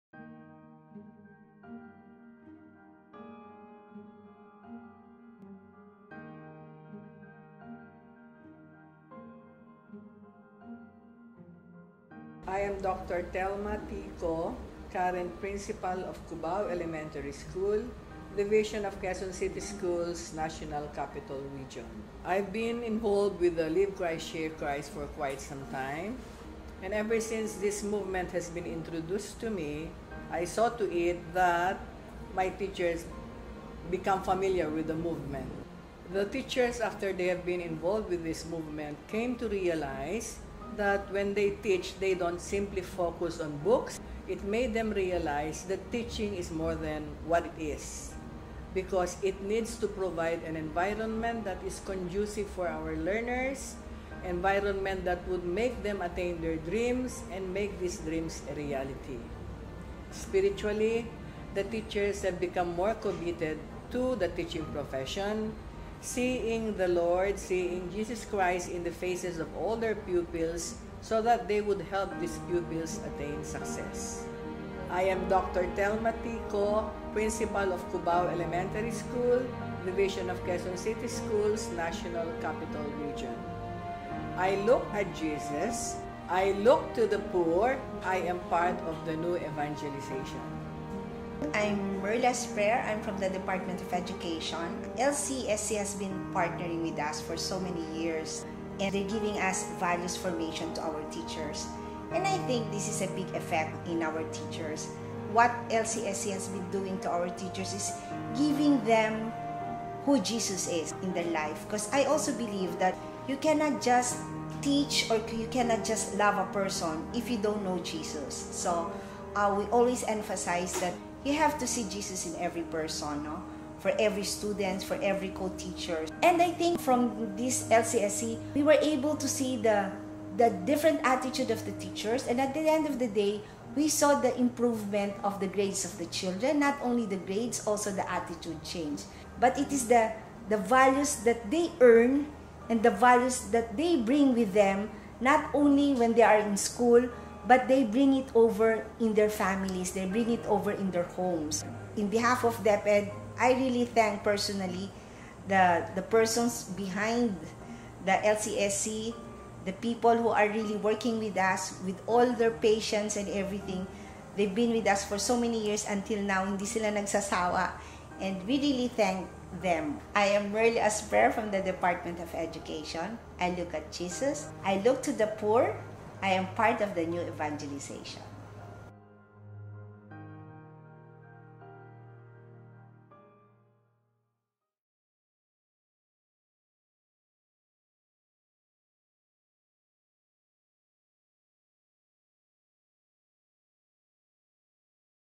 EDUCATION
Thank you for participating and making the Catholic faith alive for all to see, in the New Evangelization Conference 2015.

Christ is beginning to reach diverse people in unexpected places. Let us be reminded and inspired by how the Holy Spirit has moved in the lives of these men and women from the field of education.